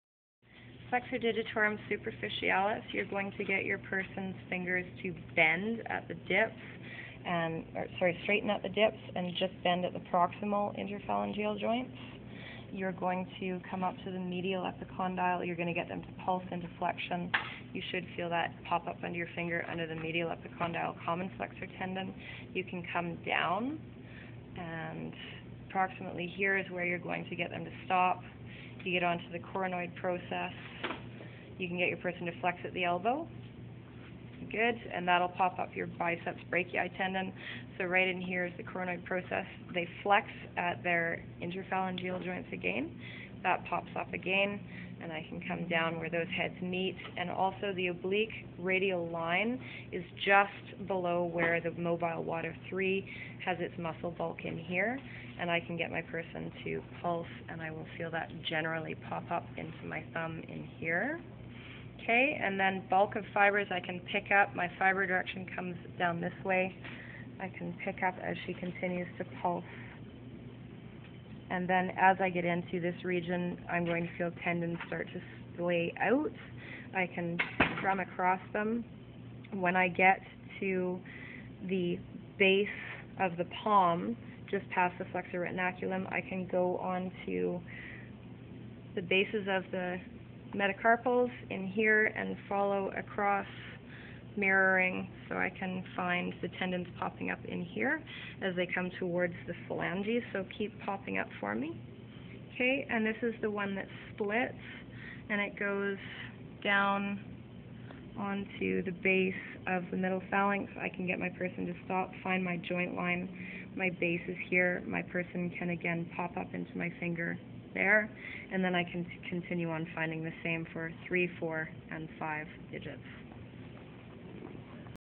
Palpation - Flexor Digitorum Superficialis
Flexor Digitorum Superficialis

Origin: Humeroulnar head- medial epicondyl of the humerus and coronoid process of the ulna   Radial head- oblique line of the radius (anterior surface)
Insertion: Shaft of the muddle phalanges of the medial 4 digits (2-5)
Nerve: Median nerve (C5-T1)
Angiology: Radial and ulnar arteries
Action: Flexes MCP and PIP joints of the medial 4 digits, and assists with wrist flexion